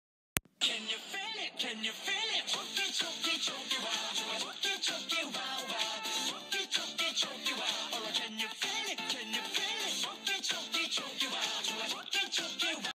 Chok Chok | Meme | Happy 3rd B-Day Baby Bro
This is a birthday special for my baby bro bro and it is just for him because he loves dogs so instead of putting in a person, I put in a dog